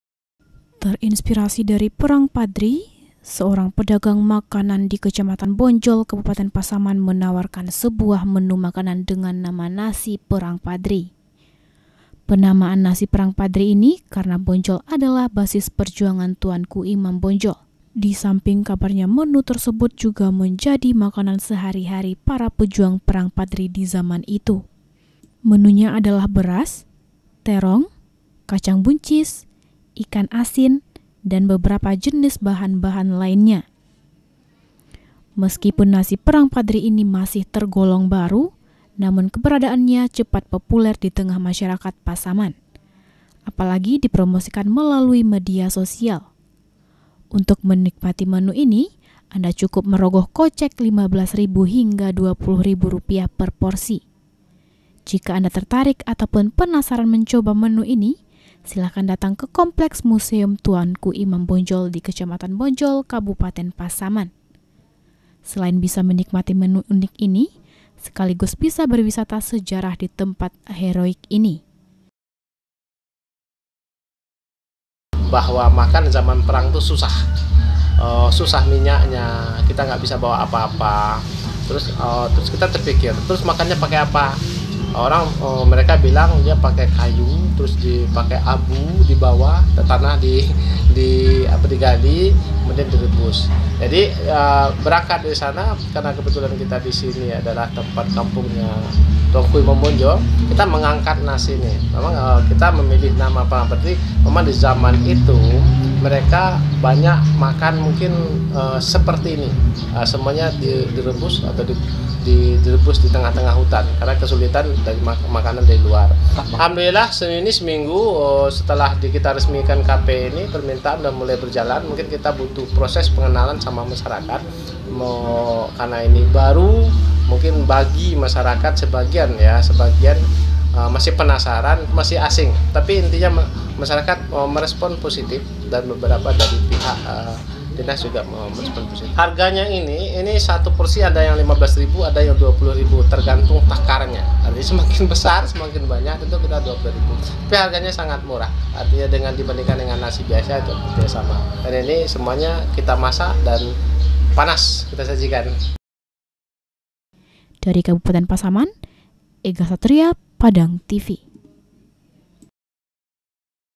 Nasi Perang Paderi Menu Makanan Yang Populer Di Sekitar Kompleks Museum Tuanku Imam Bonjol Pasaman
Siapa Yang Tidak Kenal Dengan Perang Paderi Sebuah Pertempuran Melawan Penjajah Yang Dipimpin Tuanku Imam Bonjol. Namun Perang Paderi Yang Satu Ini Adalah Sebuah Menu Makanan Yang Populer Di Sekitar Kompleks Museum Tuanku Imam Bonjol Kabupaten Pasaman. Seperti Apa Nasi Perang Paderi Itu Berikut Liputannya.